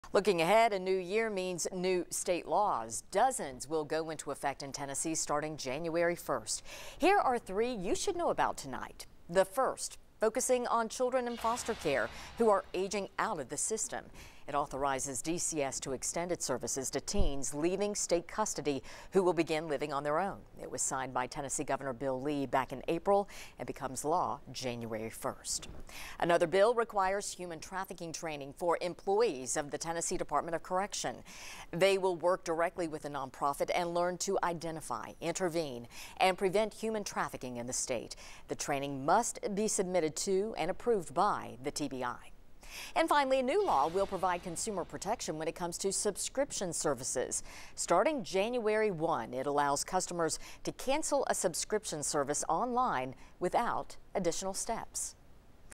Here are some of the laws that will go into effect on Jan. 1, 2023 in Tennessee
Some of the laws add training requirements for security guards and specifies that building codes need to include ways to mitigate structural collapses.